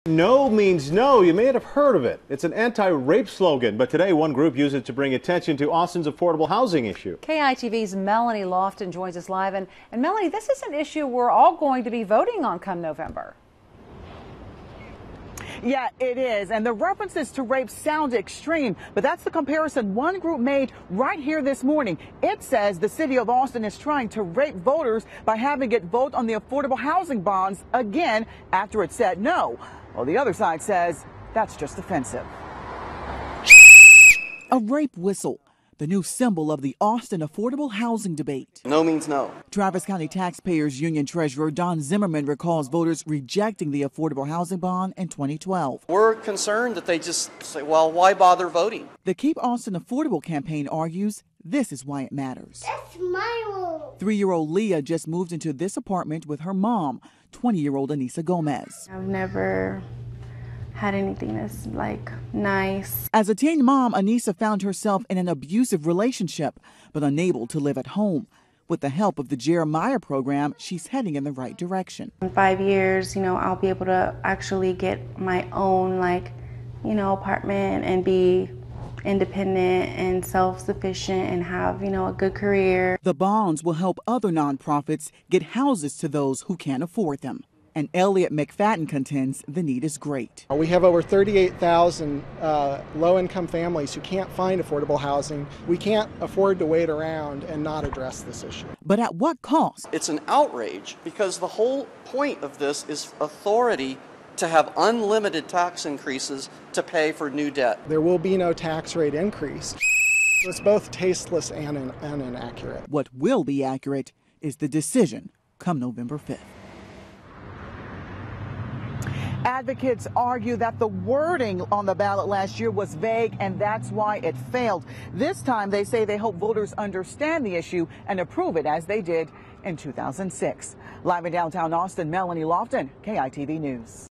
Rape Whistle Used In Affordable Housing Debate
It's an anti-rape slogan. But Tuesday one group used it to bring attention to Austin's affordable housing issue.